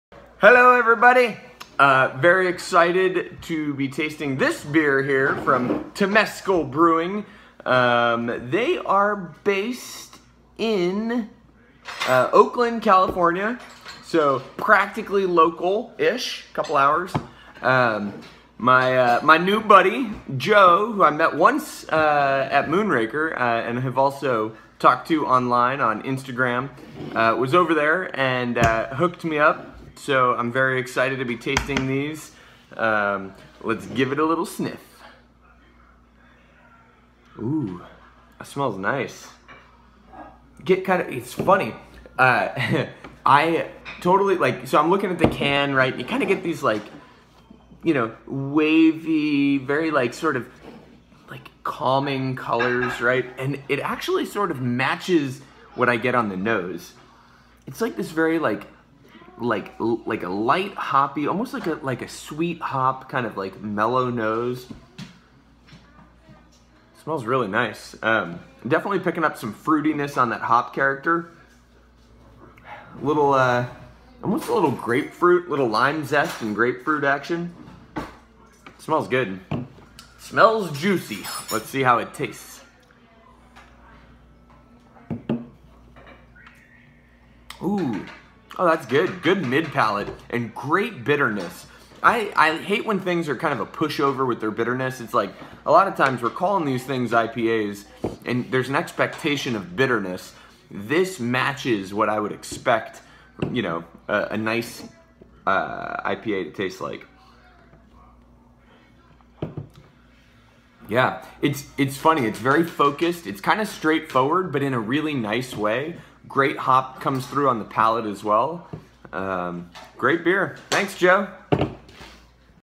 Beer Review - Post Coast by Temescal Brewing
Tasty brew from a low profile brewery in Oakland, CA.